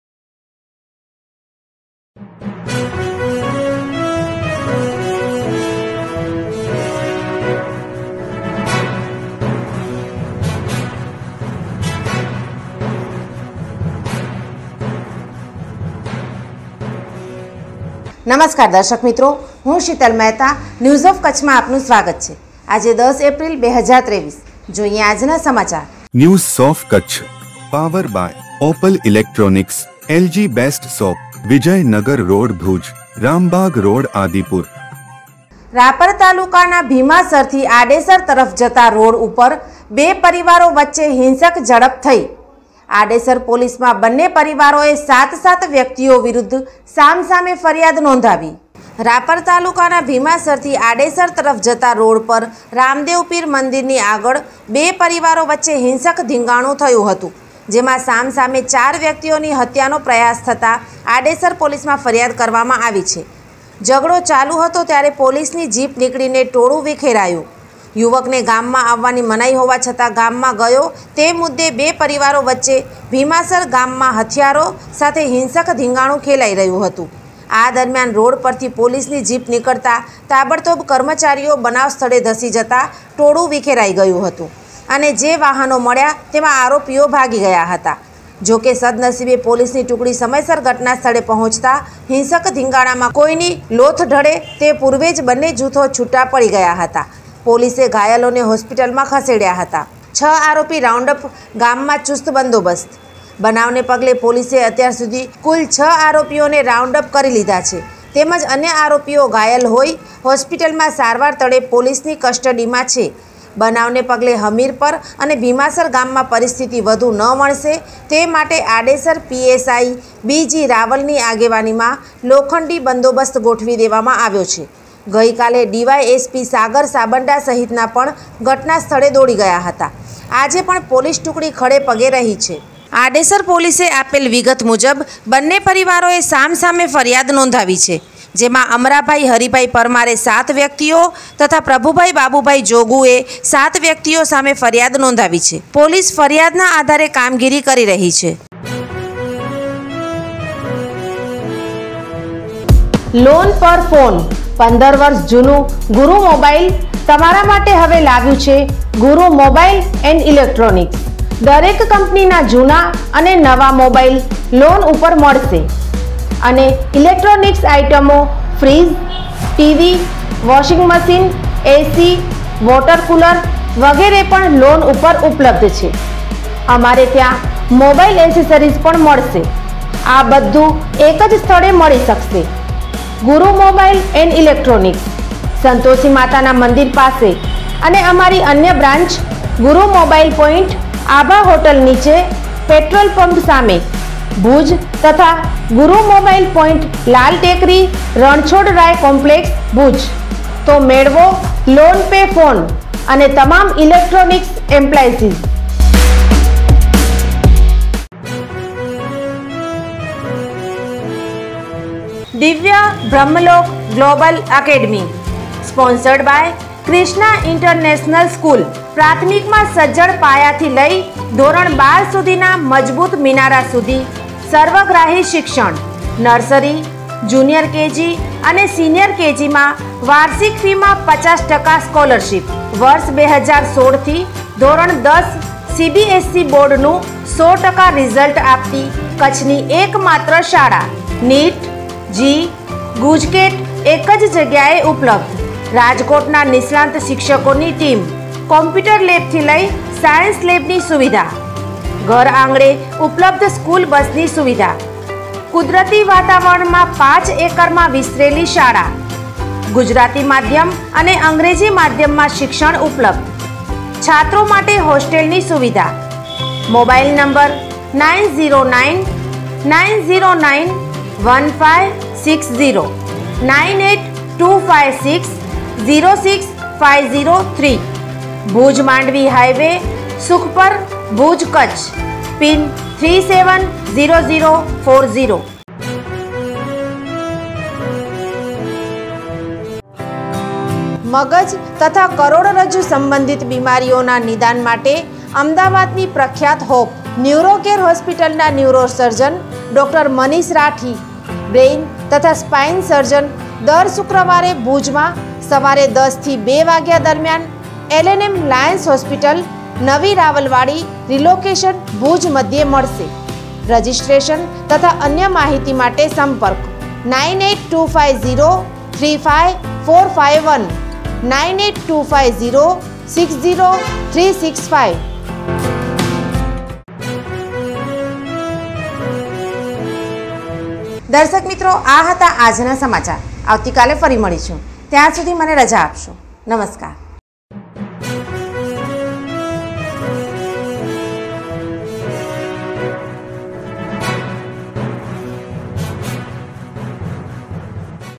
FIR launch Adisar police station રાપર તાલુકામાં ભીમાસર આડેસર વચ્ચે બે પરિવાર વચ્ચે ધીંગાણું સામે🌹
રાપર તાલુકામાં ભીમાસર આડેસર વચ્ચે બે પરિવાર વચ્ચે ધીંગાણું થતા સામ સામે ફરિયાદ થઈ અનેક ઘાયલો ને હોસ્પિટલમાં સારવાર માટે દાખલ🌹